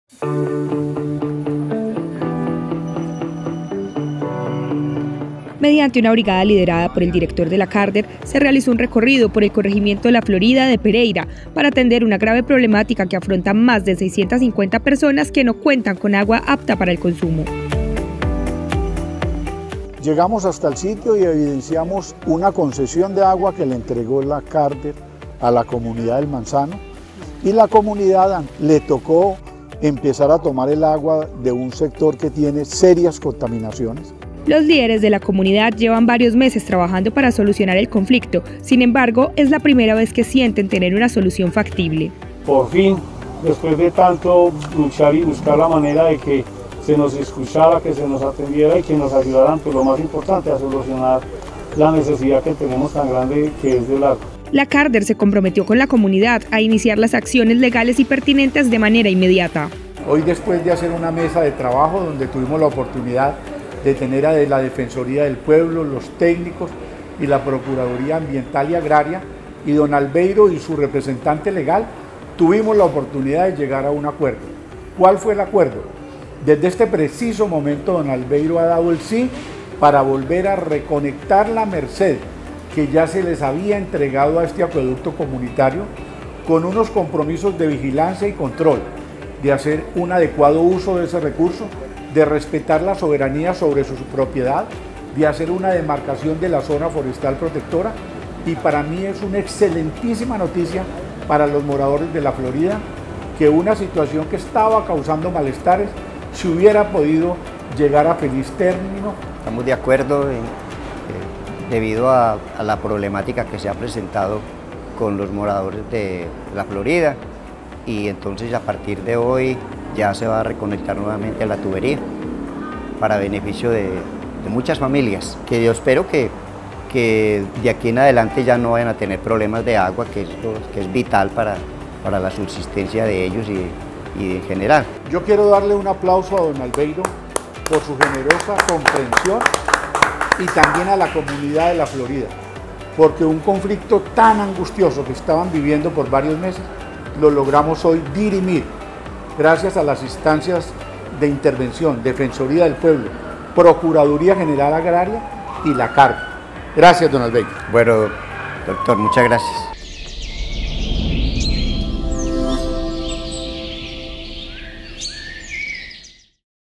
Superado conflicto por agua en el corregimiento de La Florida, en Pereira
La Cárder lideró una importante brigada con el apoyo de otras entidades municipales para resolver una problemática frente al abastecimiento de agua potable para más de 650 personas del corregimiento La Florida de Pereira, gracias a esta gestión la comunidad contará con el servicio y la tubería será reconectada.